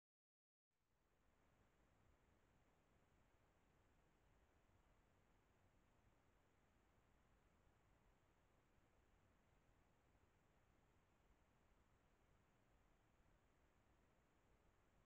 Is het een das of een vos op de video
Links onderin loopt vermoedelijk een das te scharrelen... of is het een vos?